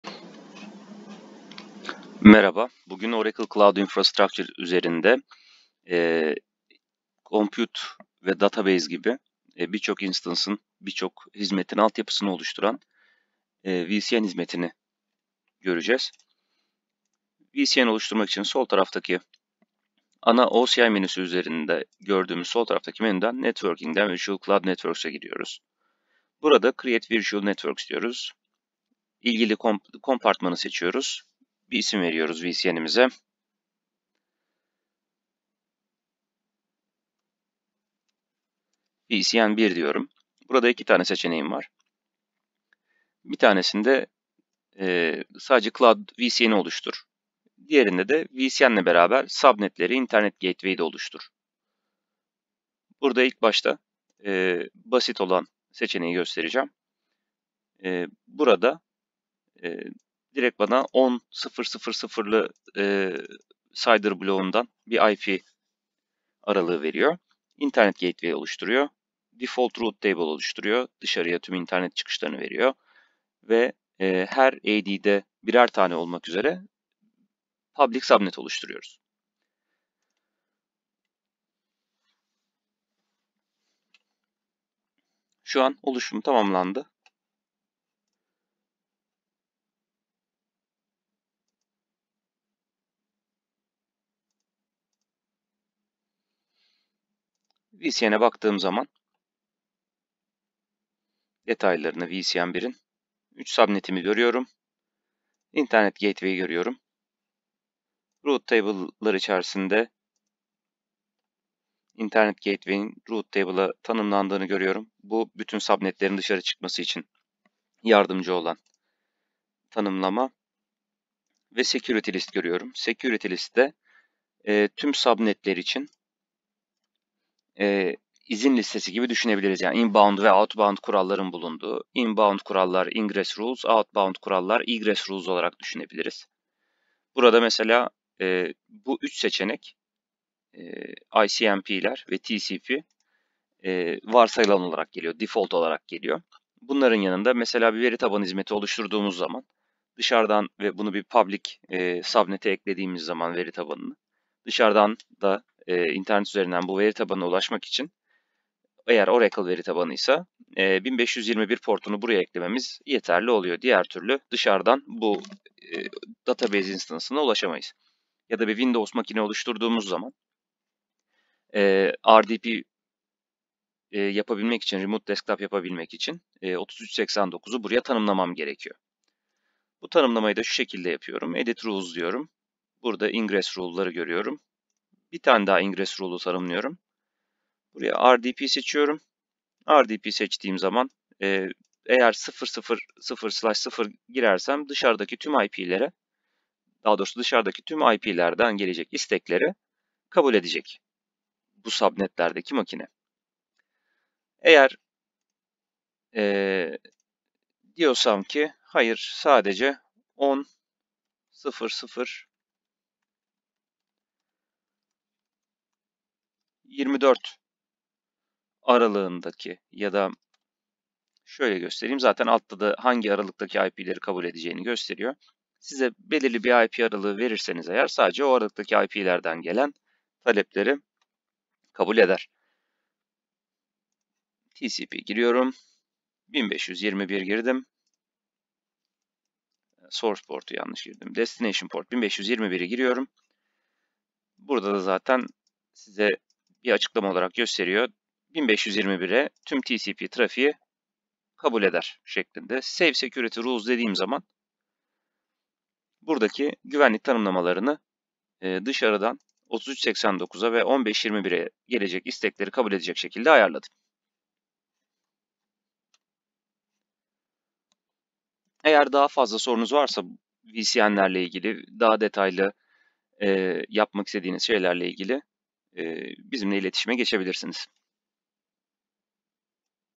[TÜRKÇE] Oracle Cloud Infrastructure - Virtual Cloud Network oluşturulması
Merhaba,

Bugün, mümkün olduğu kadar basit bir anlatımla Oracle Cloud Infrastructure üzerinde en temel hizmetlerden biri olan VCN oluşturulmasını konu aldım.
Sorularınız varsa yorumlara ekleyebilirsiniz.